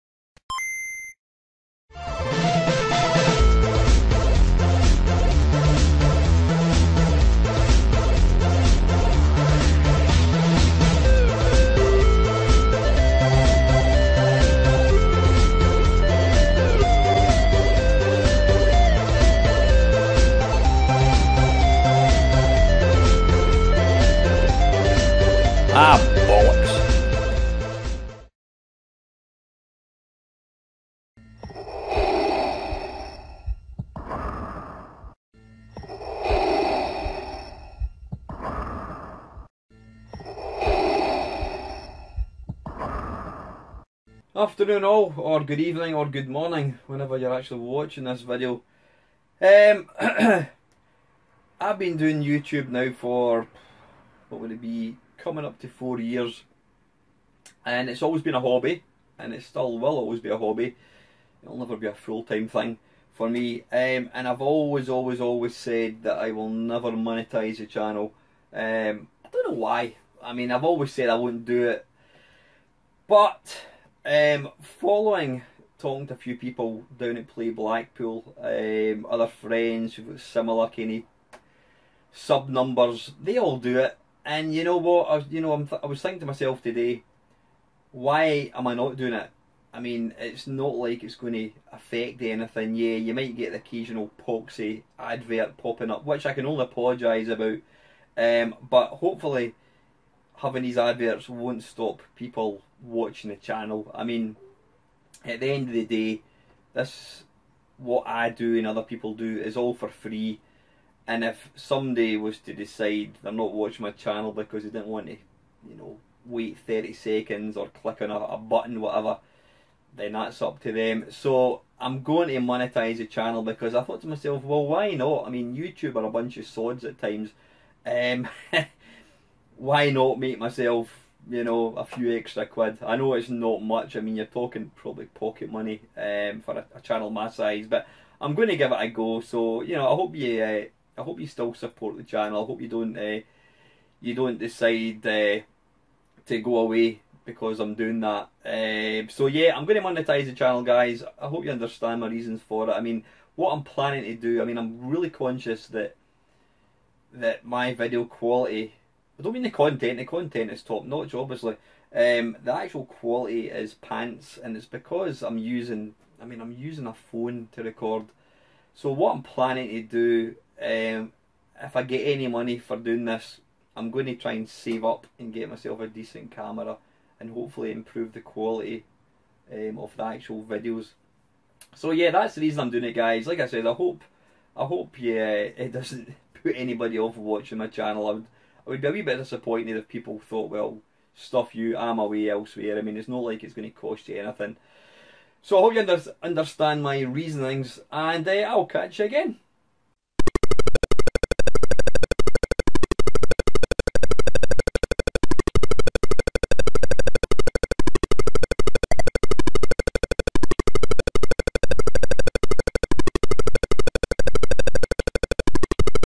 Turning to the Dark Side - Monetising The Channel
If You Tube want to throw pocket money my way I'd be a fool to not take it!